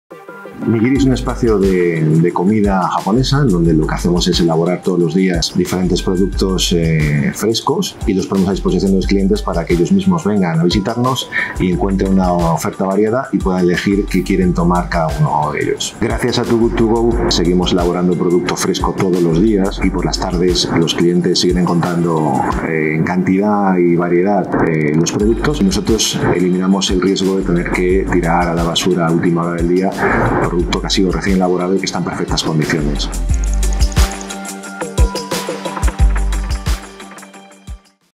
Nigiri Lucha Contra el Desperdicio de Alimentos - Too Good To Go España
Víctor Anaya, propietario de los restaurantes de comida japonesa Nigiri, explica su colaboración con TooGoodToGo para luchar contra el desperdicio alimentario, porque

Descárgate ya nuestra app y lucha contra el DesperdicioDeAlimentos

¡Descubre nuevos consejos y noticias sobre el Food Waste en nuestras redes!